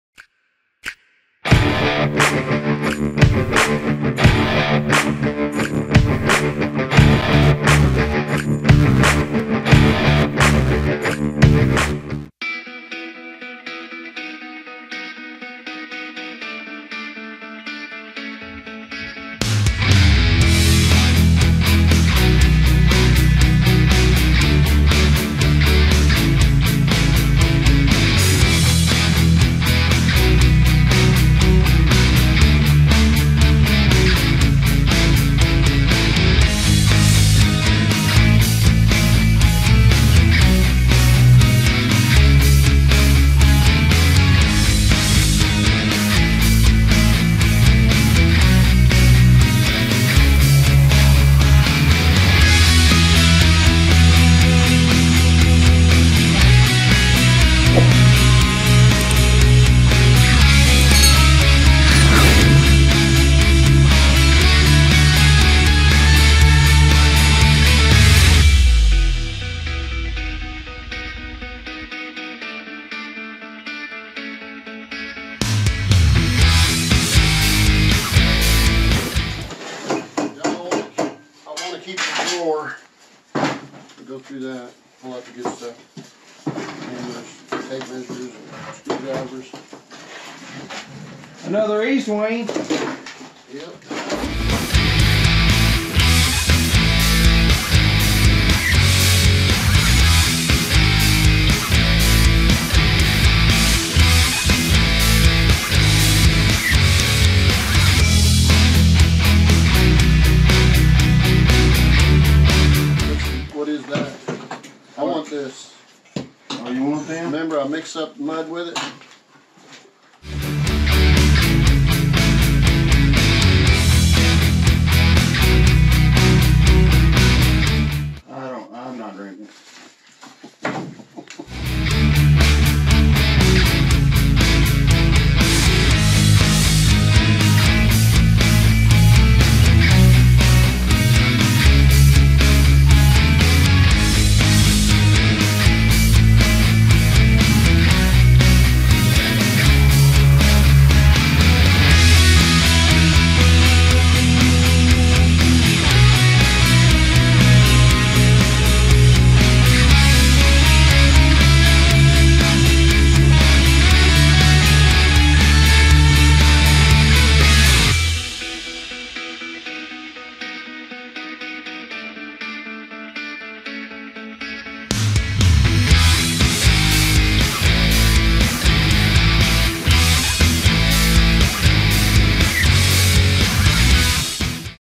Mobile Home Hoarders Clean Up We're Inside
We finally made it completely inside the trailer. The guys are shoveling the trash out. Crazy

⏱️⏱️Chapters⏱️⏱️
00:00 Intro
00:12 Shoveling the trash out of the kitchen
01:20 Saving a drawer
01:54 We use old mixer blades to mix drywall mud
02:07 Drink anyone?
02:52 Follow us